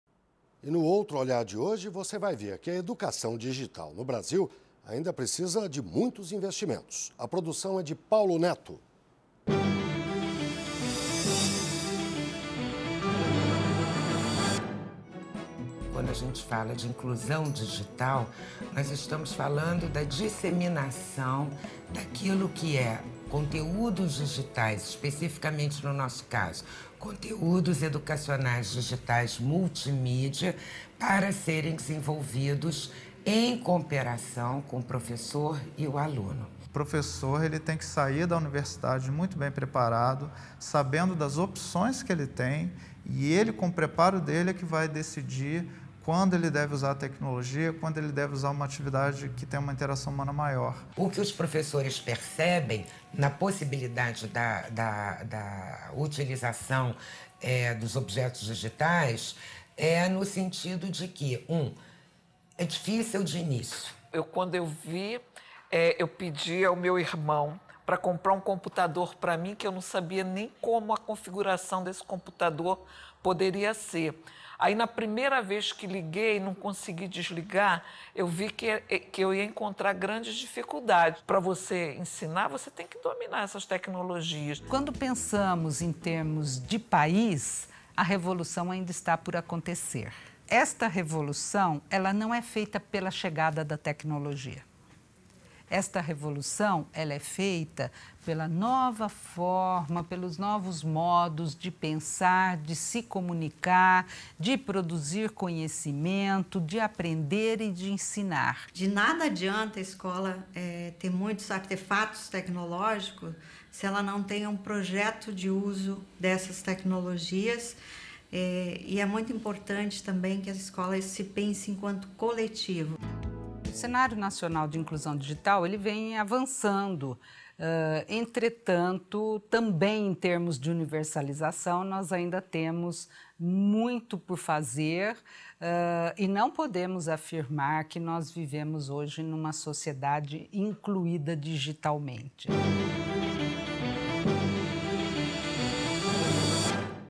OUTRO OLHAR - Educação digital
Você vai ver que a educação digital no Brasil ainda precisa de muitos investimentos. A produção é de Paulo Netto.